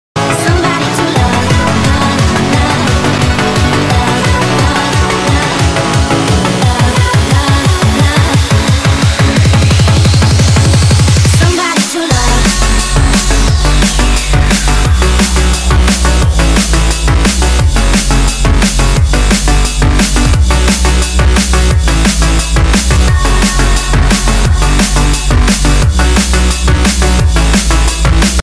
Denis - BSTC p1 QUATER FINAL
This is my combo for the competition. Thanks to my girlfriend for the motivation!!! (h)